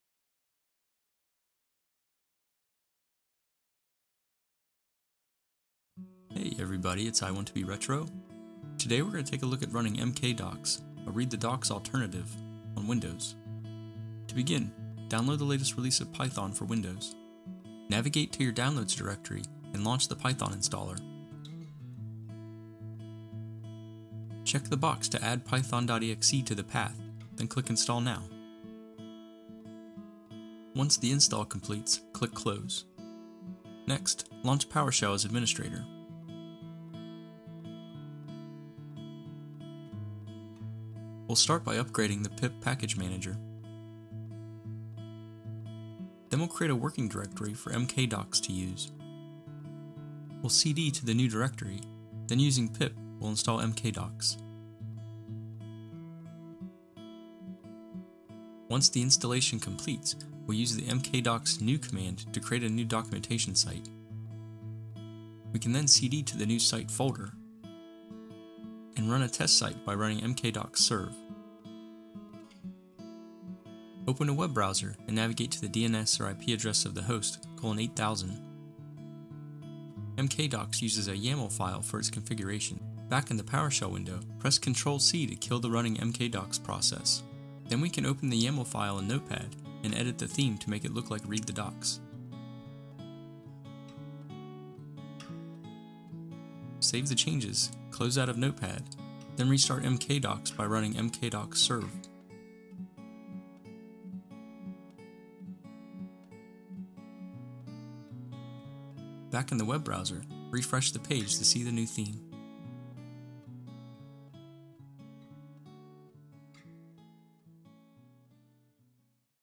Install MkDocs - Read the Docs Alternative - on Windows
What is MkDocs?
MkDocs is a fast, simple and downright gorgeous static site generator that's geared towards building project documentation. Documentation source files are written in Markdown, and configured with a single YAML configuration file. It is designed to be easy to use and can be extended with third-party themes, plugins, and Markdown extensions.

Installation
   01. Log into the Windows device
   03. Run the downloaded Python installer, making sure to check the option to add Python to the path
   04. Launch PowerShell as Administrator
   05. Run the following commands in PowerShell
         # upgrade pip
         pip install --upgrade pip
         # create a mkdocs directory in user directory
         mkdir $ENV:UserProfile\mkdocs
         # change directory to mkdocs
         cd $ENV:UserProfile\mkdocs
         # install mkdocs
         pip install mkdocs
         # create a mkdocs site
         mkdocs new mkdocs-demo
         # change directory to mkdocs-demo
         cd mkdocs-demo
         # start the demo site
         mkdocs serve
06.

Read the Docs Theme
   01. Back in PowerShell, press CTRL+C to kill the running process
   02. Run the following command
         # open the mkdocs config file in notepad
         notepad "$ENV:UserProfile\mkdocs\mkdocs-demo\mkdocs.yml"
   03. Add the following line to mkdocs.yml
         theme: readthedocs
   04. Save the change and close Notepad
   05. Run the following command to see the change
         # restart the demo site
         mkdocs serve